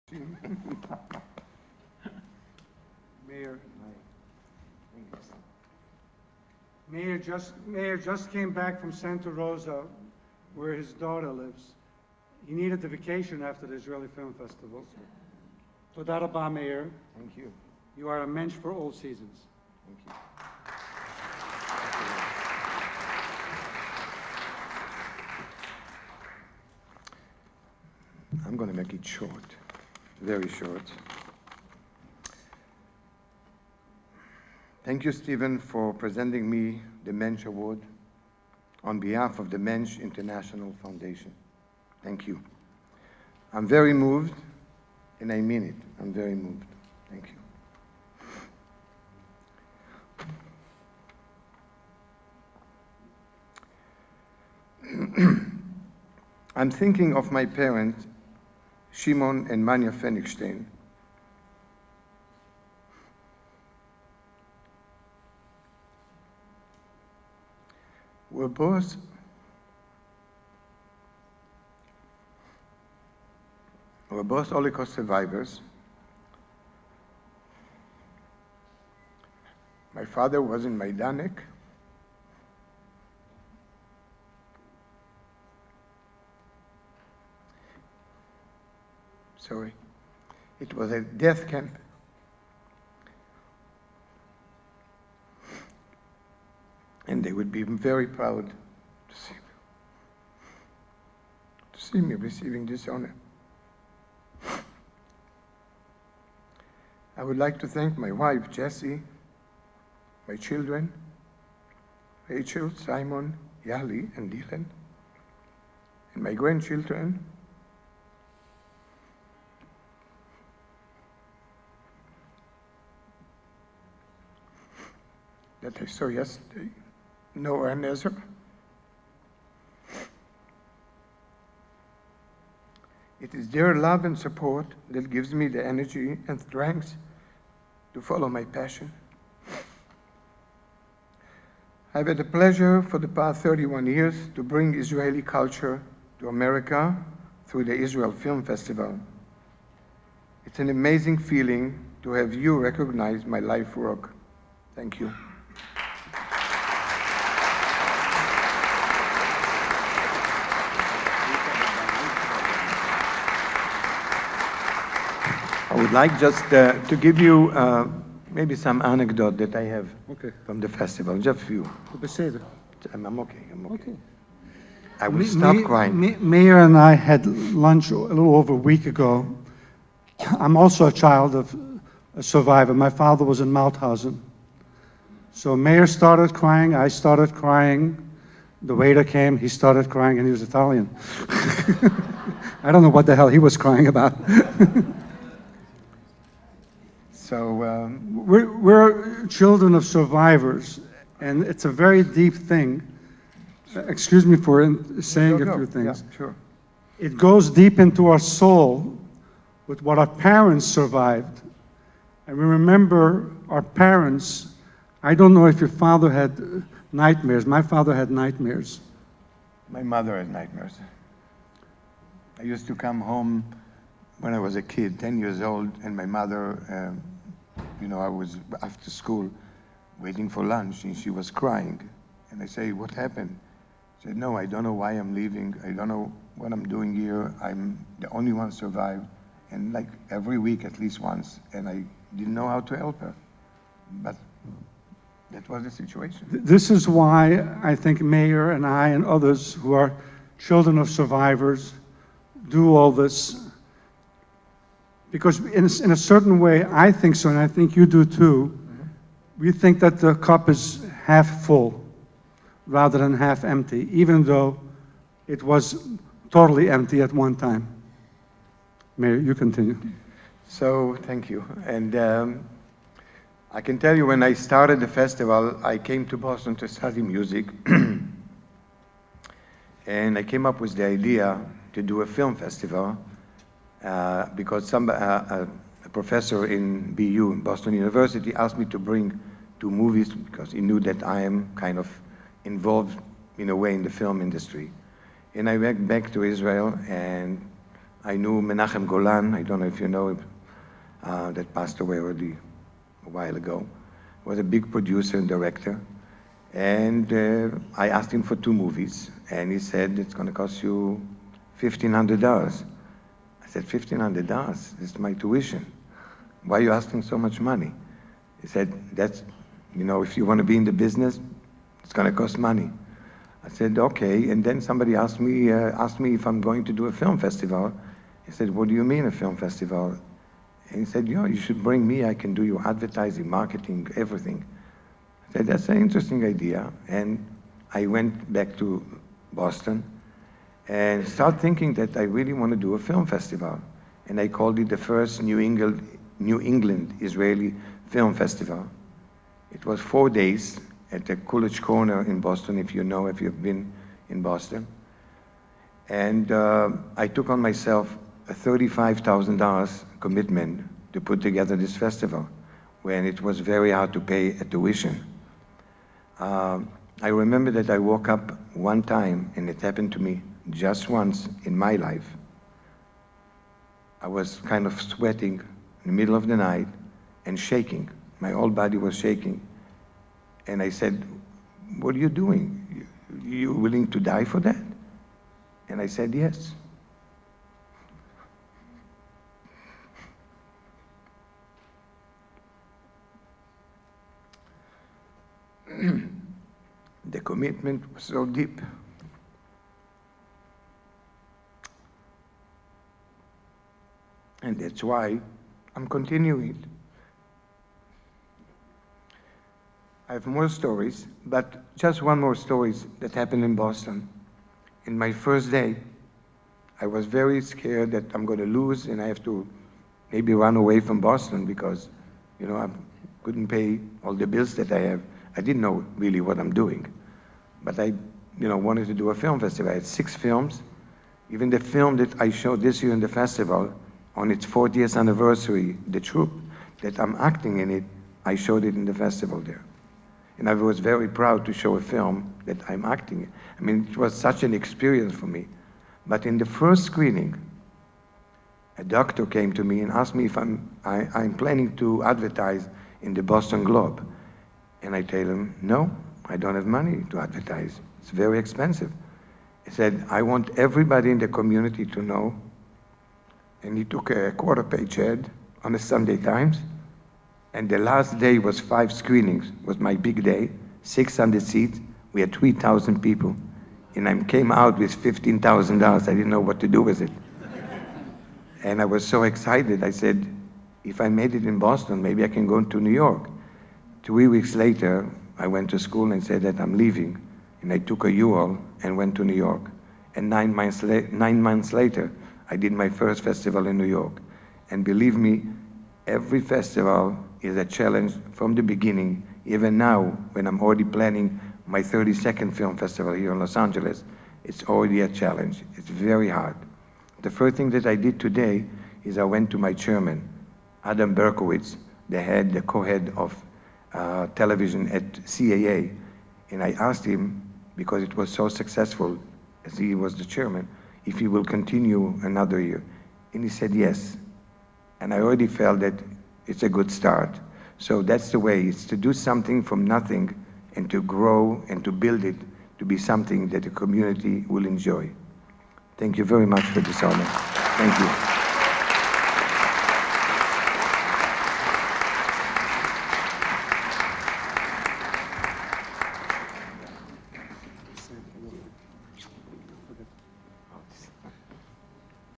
Steve Geiger presents Mensch-Award to Shoah-survivor son, Meir Fenigstein (Israel Film Fest)
Mensch Int'l Fndn Exec Director, Steven Geiger, honors Meir Fenigstein of Israel Film Festival with a "Mensch Award" on the night celebrating the 70th anniversary of Israel's founding via the U.N. Partition of Palestine Plan, Nov 29, 1947. Event with actor Mike Burstyn, was held at Sinai Temple, Los Angeles, California.